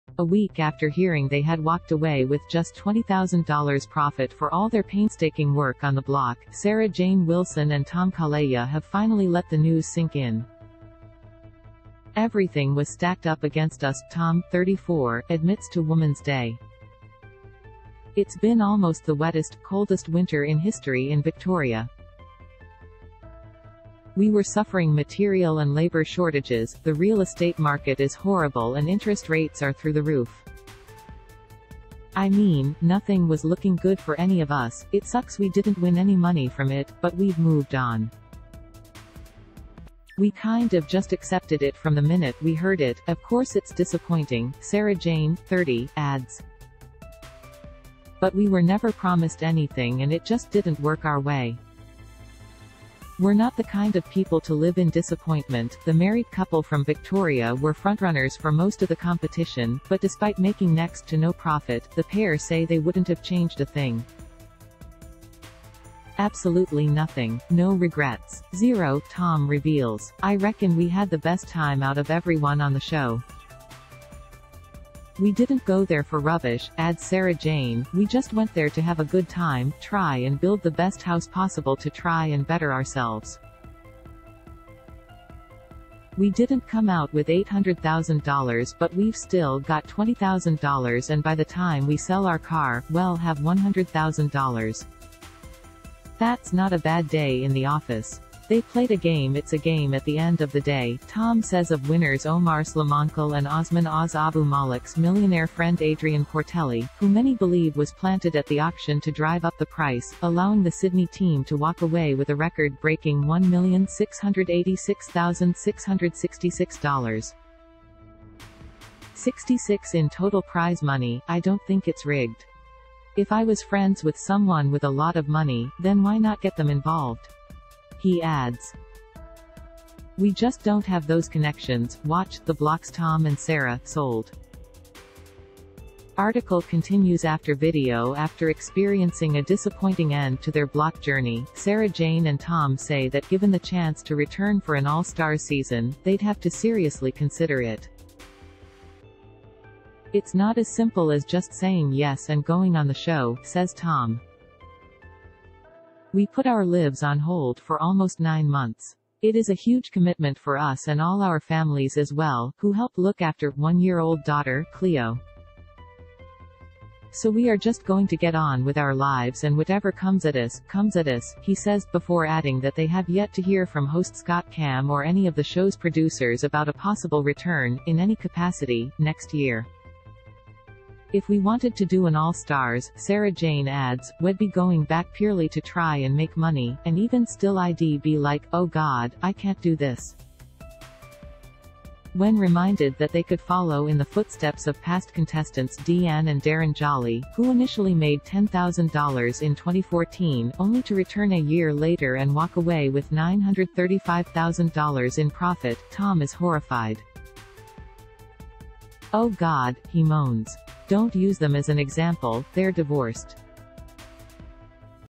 EXCLUSIVE: The Block’s Tom and Sarah-Jane speak out: ‘We’ll return on our terms’
A week after hearing they had walked away with just $20,000 profit for all their painstaking work on The Block, Sarah-Jane Wilson and Tom Calleja have finally let the news sink in."Everything was stacked up against us," Tom, 34, admits to Woman's Day. "It's been almost the wettest, coldest winter in history in Victoria. We were suffering material and labour shortages, the real estate market is horrible and interest rates are through the roof. I mean, nothing was looking good for any of us."It su